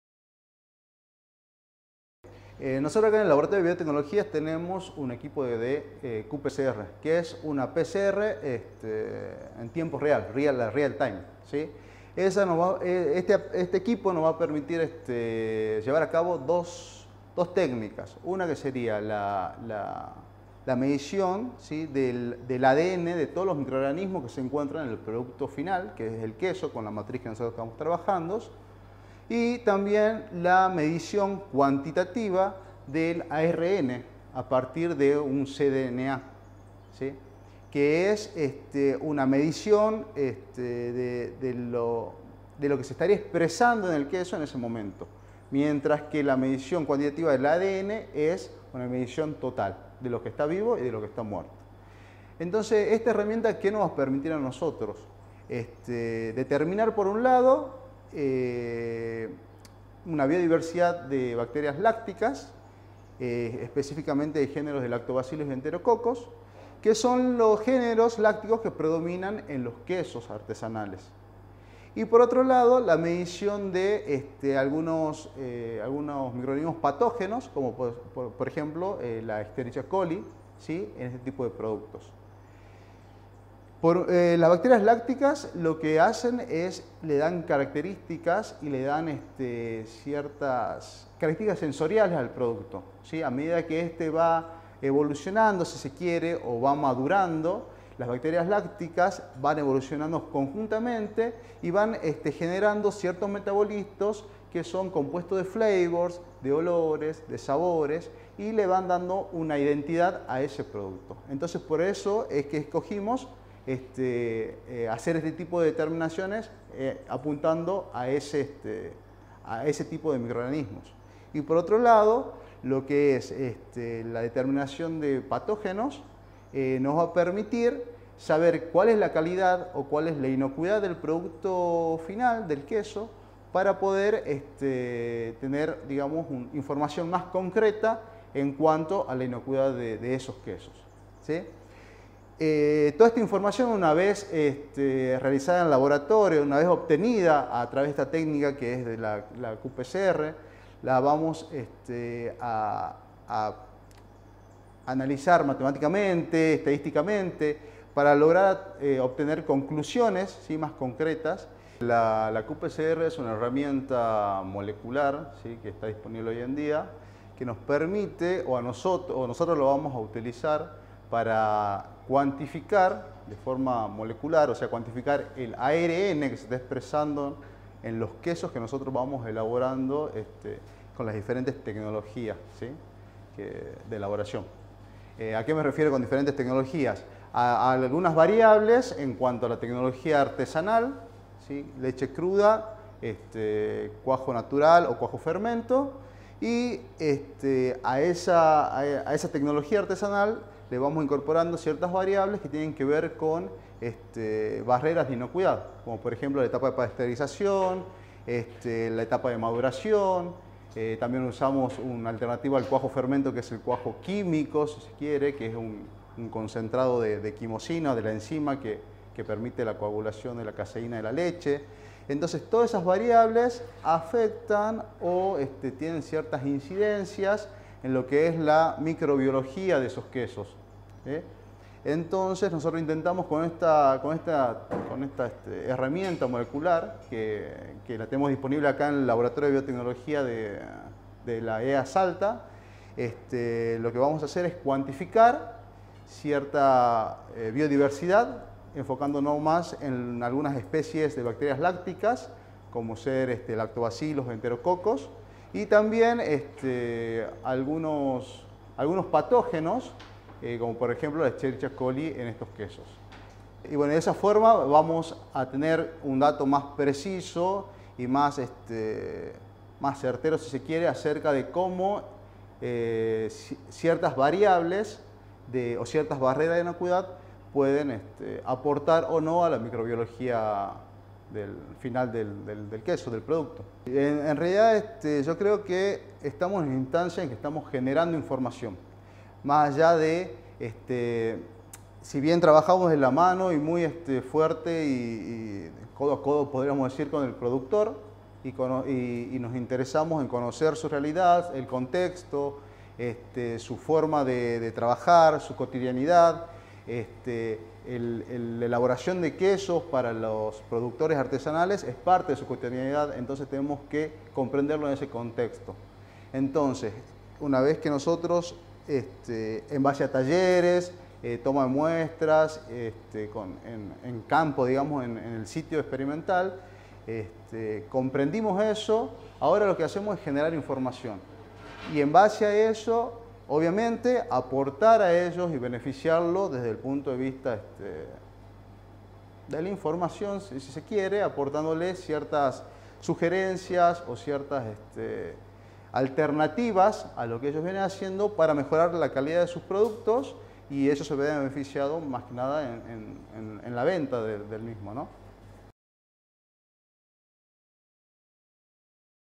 Quesos artesanales con inocuidad
Desde el laboratorio de Biotecnología de INTA Salta, se trabaja en la determinación y medición de la biodiversidad de bacterias lácticas y patógenos existentes en quesos artesanales.
El interés principal radica en elaborar un producto con inocuidad, calidad, que favorezca su comercialización y asegure la salud del consumidor. Al respecto, nos habla el Ing. Nicolás López (CONICET - INTA Salta)